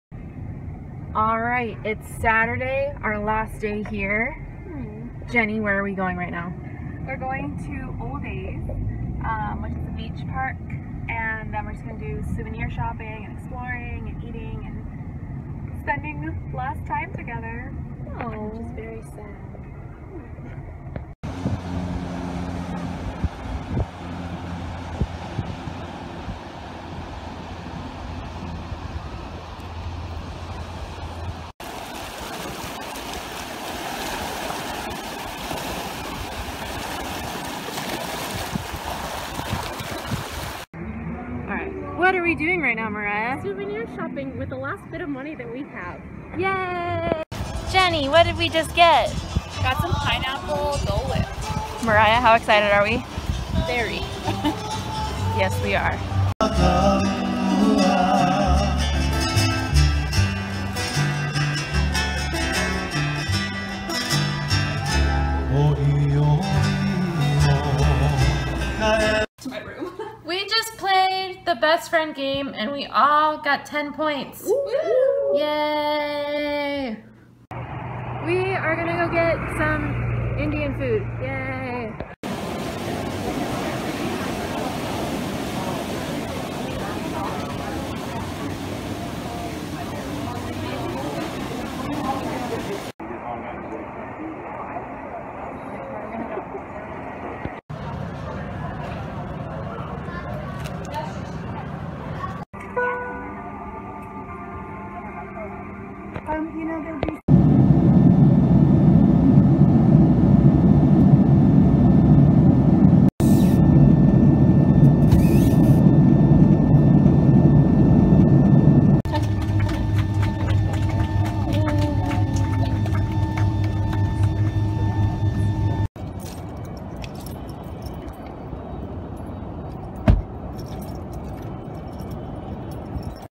Day 6/7 - Farewell Kona
Our last day in Kona, HI with Jenny! We went to one last beach, walked around downtown Kona, ate pineapple Dole Whip, and saw some Hula dancers before the ocean decided to splash us with water. One last red-eye flight back to Phoenix brought us home.